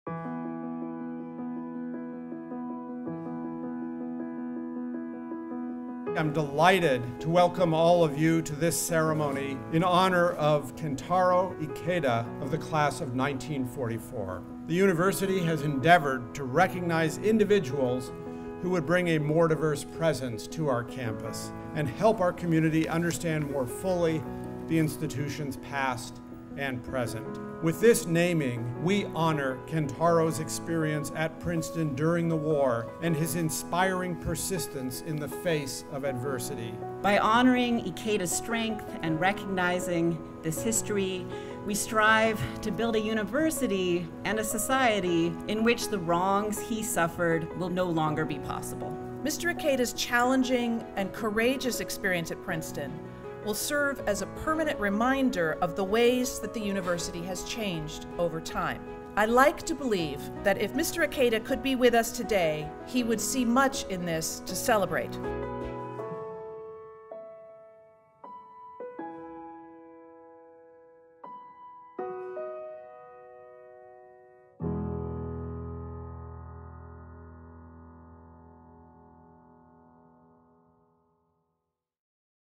Princeton names campus arch for Kentaro Ikeda ’44
Princeton names campus arch for Kentaro Ikeda ’44, the University’s sole Japanese student during World War II

Ikeda was born in Kanazawa, Japan, and moved to New Jersey in 1938 to attend boarding school. During his sophomore year at Princeton, Japan attacked Pearl Harbor and the United States entered World War II. In February 1942, the U.S. issued an executive order authorizing the government to incarcerate people of Japanese descent, including U.S. citizens, in internment camps.

Ikeda worked with the federal government and the University to avoid deportation and imprisonment, but was essentially confined to Princeton. He was prohibited by wartime restrictions from leaving town without permission, from accessing funds and from communicating with anyone in Japan, including his family.

After the war ended, Ikeda stayed in the U.S. with support from American friends and from Princeton administrators and others who helped him gain a visa extension. Later, he was able to legally immigrate to the U.S. and remained in the country for the rest of his life. He was a well-known tea importer in his family’s business and a longtime resident of Larchmont, New York.